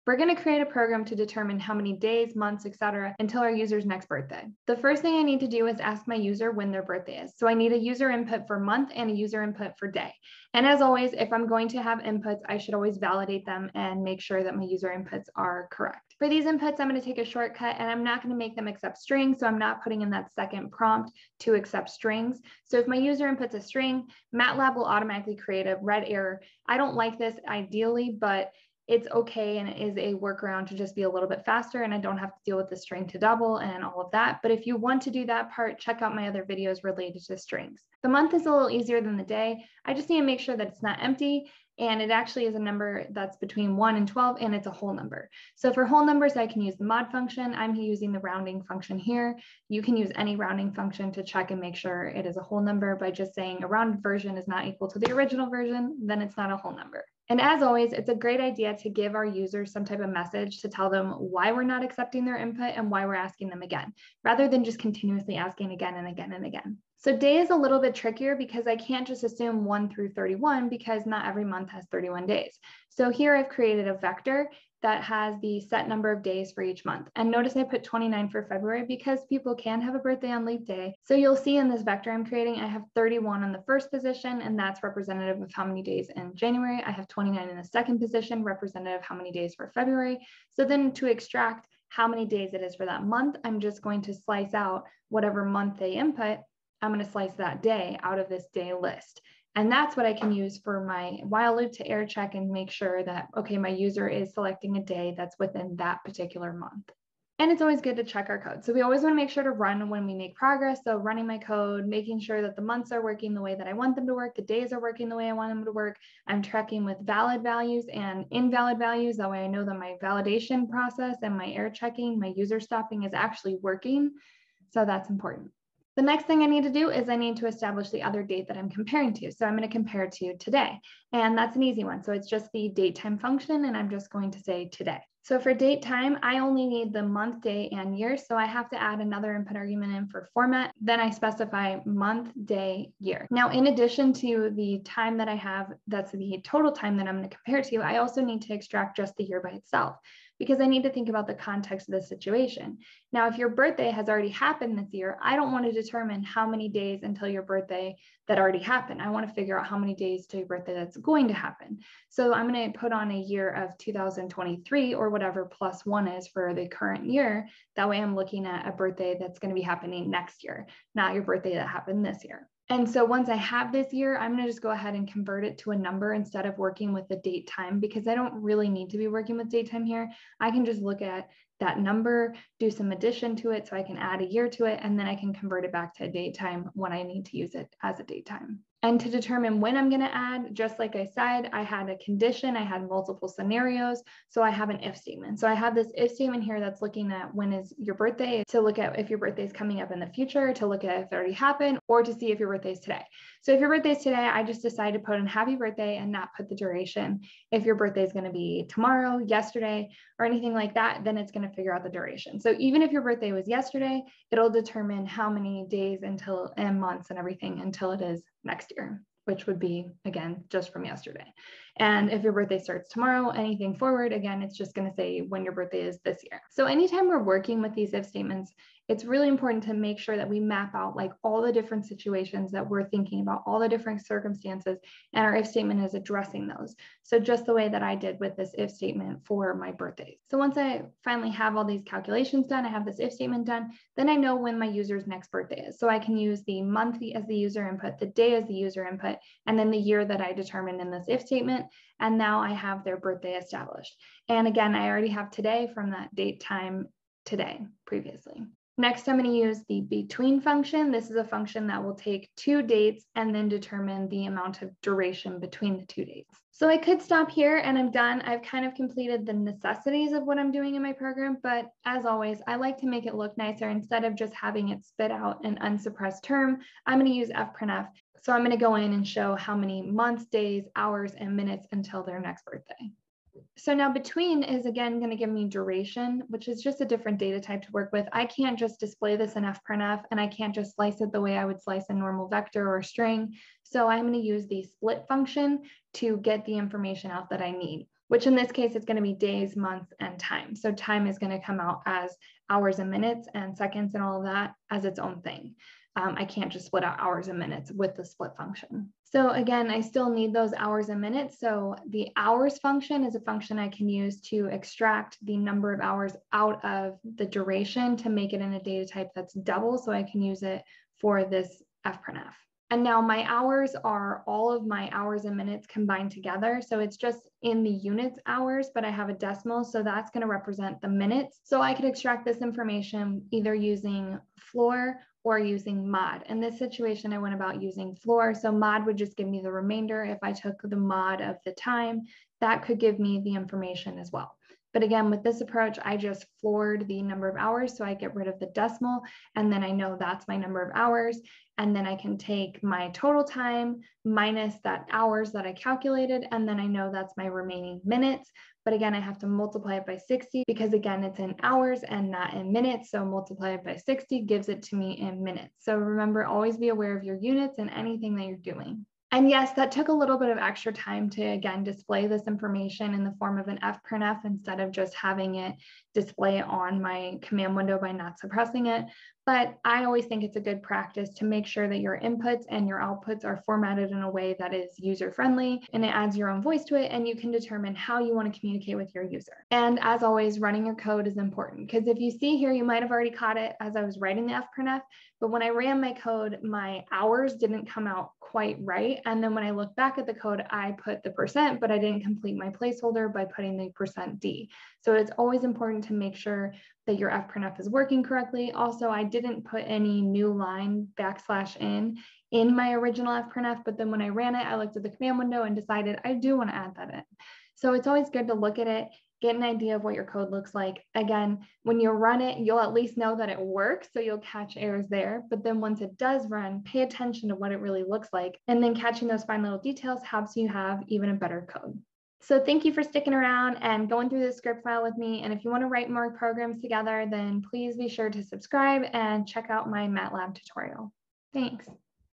MATLAB: Calculate time to birthday/event (datetime and between functions)
MATLAB date related functions: duration between two dates

How to collect an input from a user and validate it. Collecting the user's birthday month as a number. This is a first step to a MATLAB program to calculate how long until someone's next birthday using date functions.

MATLAB function datetime used to find the date and time for right now or today.

MATLAB function datetime used to find the date and time. Change a date datatype to a double so you can use it as a number. MATLAB functions used to convert data: char() and str2double().

MATLAB function between used to find the duration between two different dates.

datetime('now')
datetime('today')
datetime('yesterday')
datetime('tomorrow')

Chapters:
00:00 Introduction
00:04 User inputs and validating them (month and day)
01:47 Test code frequently
02:05 Today's date (datetime function)
02:23 Year of user's next birthday explanation
02:52 Convert datetime data to double (numeric)
03:08 If statements (year of user's next birthday)
04:00 Convert double data to datetime
04:18 Time/duration between dates (between function)
04:37 Display information in fprintf (output)
04:58 Convert duration to number of months, days, hours, & minutes
06:09 Seriously run code frequently
07:12 Thank you!